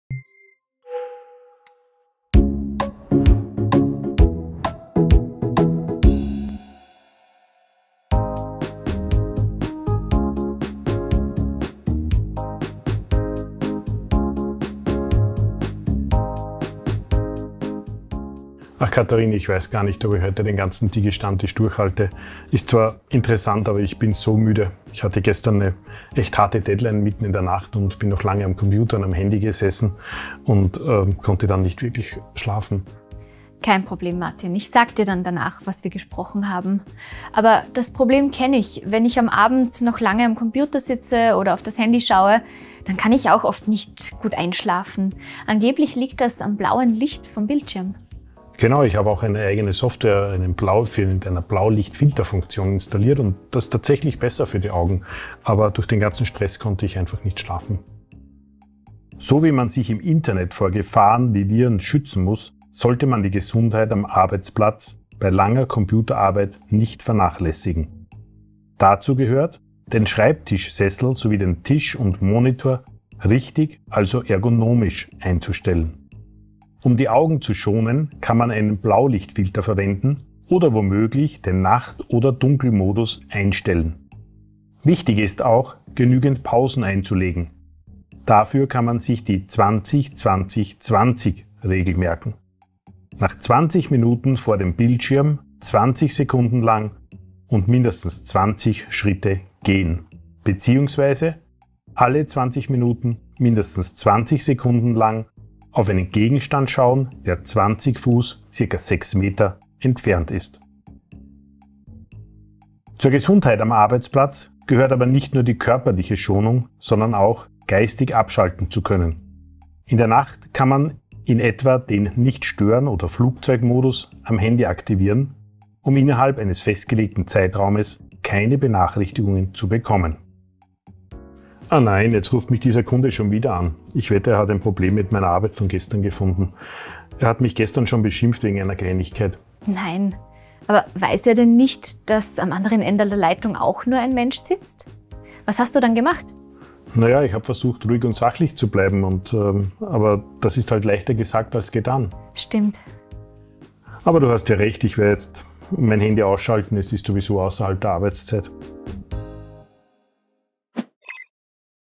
[DigiKompStudi] Gesundheit und Persönliches Wohlbefinden (4.3.1, 4.3.2)
Dies ist ein Video des Kurses "Digitale Kompetenzen für Studienanfänger:innen"